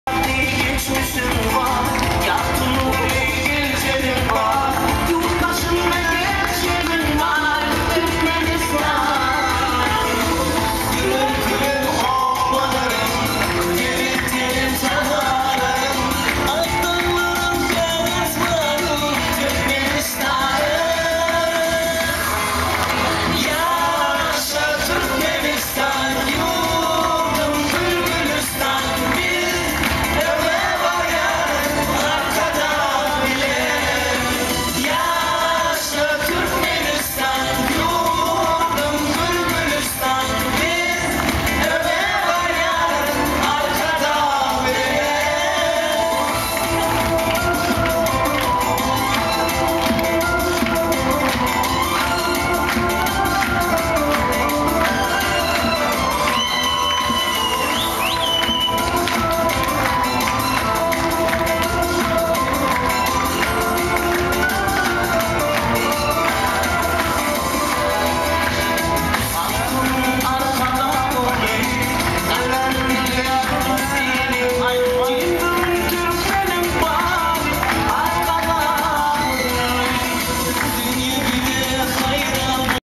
Фрагмент виступу на концерті, присвячному20-річчю нейтралітету Туркменістану, сцена Сум ДПУ ім. А.С. Макаренка, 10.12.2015.